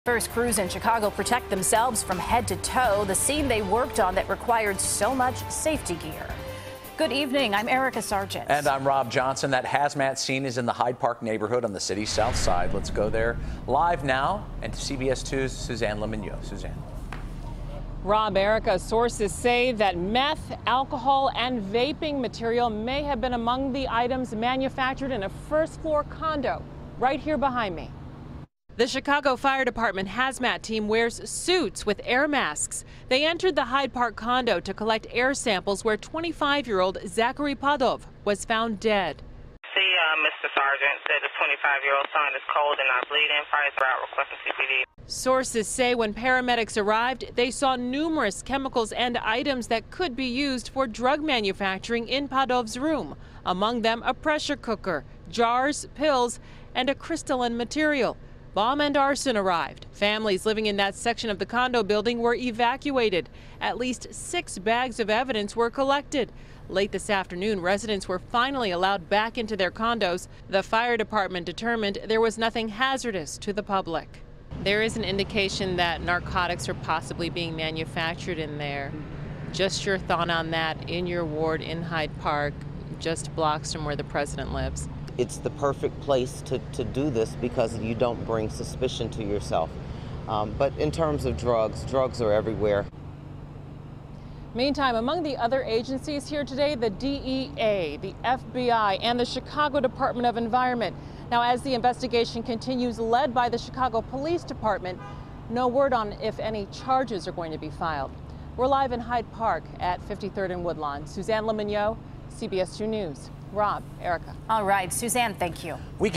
Sources: Hyde Park Apartment Where Man Died Might Have Been Meth Lab
CBS 2's Suzanne Le Mignot reports.